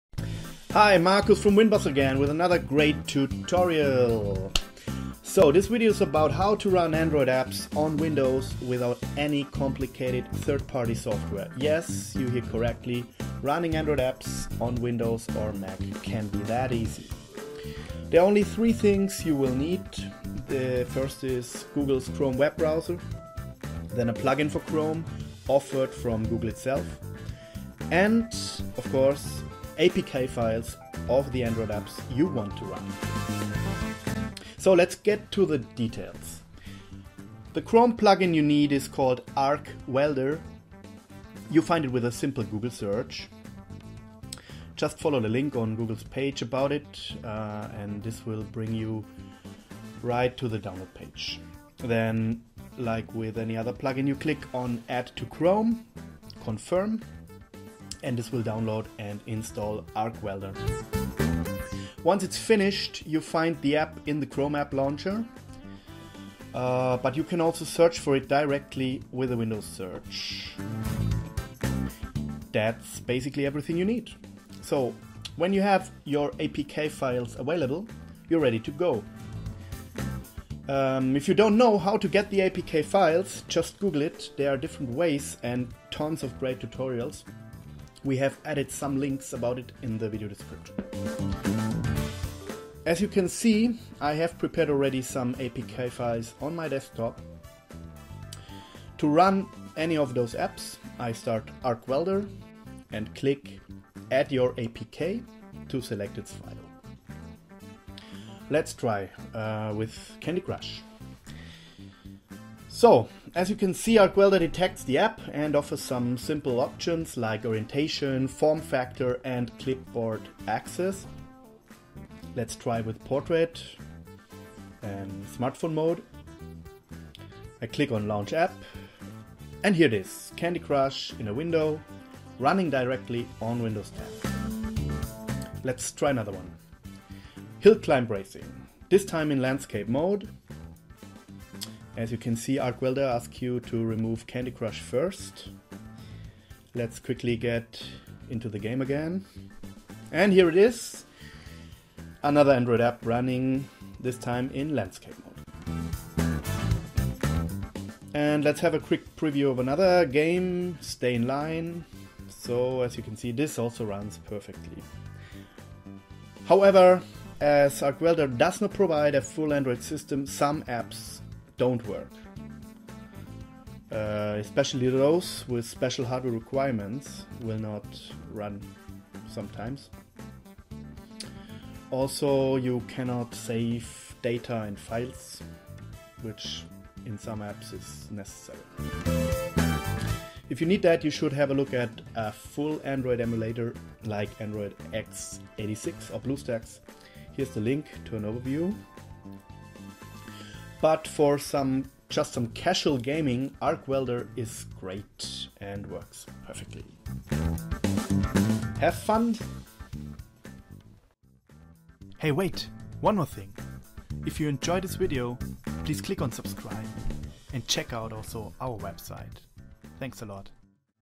Running Android apps on Windows 10: #2 ARC Welder Plugin for Google Chrome Browser
Did you know you can Android apps with Windows 10? There are even different ways to run Android apps or even a whole Android system with Windows 10.

This method runs Android apps using the ARC Welder plugin for Google´s Chrome browser. The apps are opened directly in a new window as if they were native Windows 10 apps.